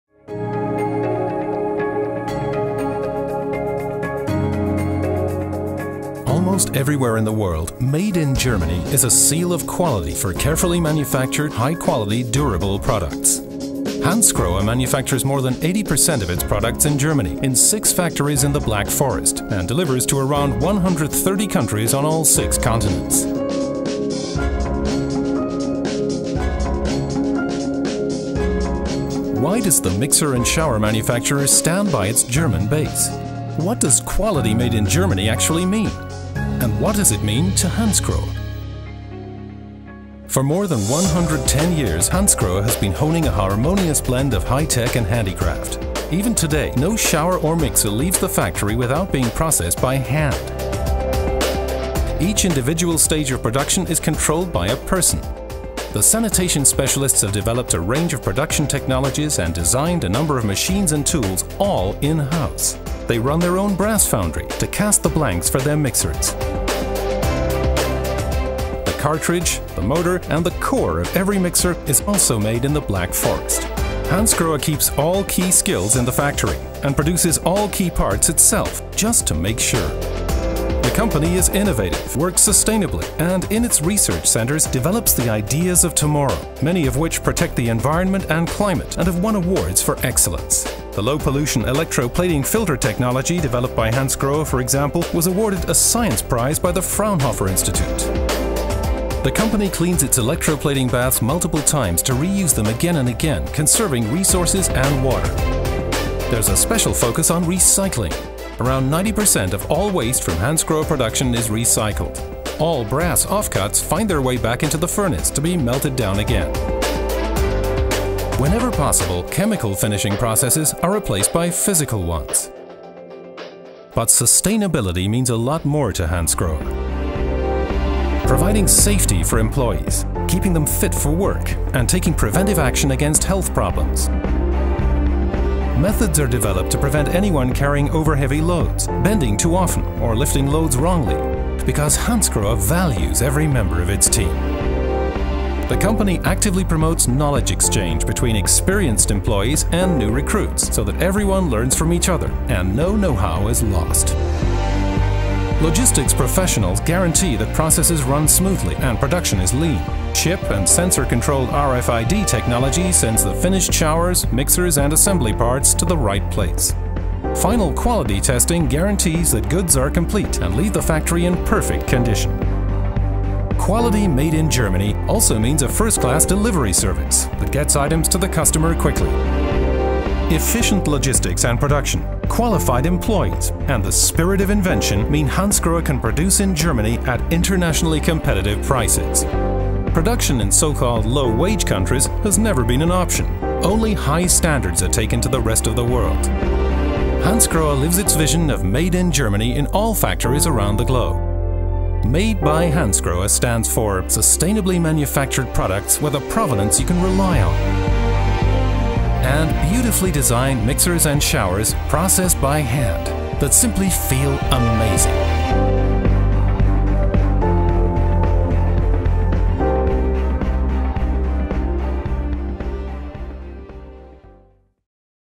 Made by Hansgrohe. Since 1901.
With more than 110 years of company history, we are committed to tradition. At Hansgrohe, this means constant development and always being ahead of the times.  Our continuous innovative thinking has helped to set trends in the world of bathrooms and kitchens, thus establishing worldwide standards and winning numerous design awards that show we are living up to our name. The best proof, however, is our many satisfied customers.

Learn more about the quality products for shower, bathtub and kitchen from Hansgrohe and Axor